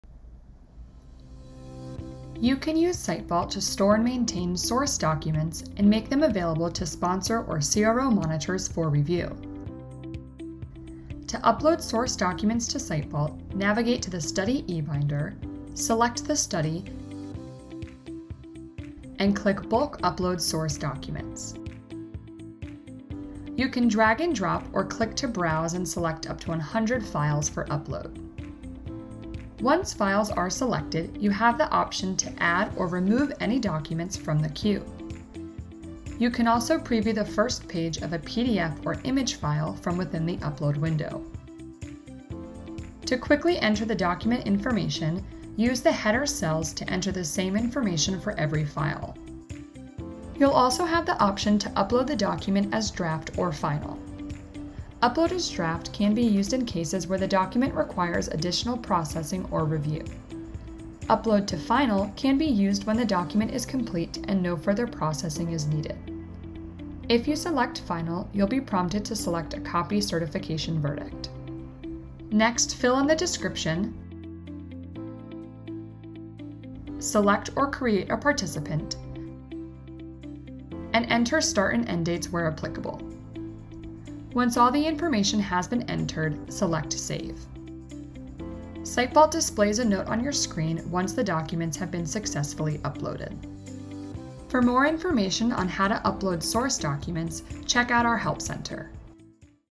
Uploading Source Documents
Learn how to upload source documents in Veeva SiteVault eReg. You can learn more in our help documentation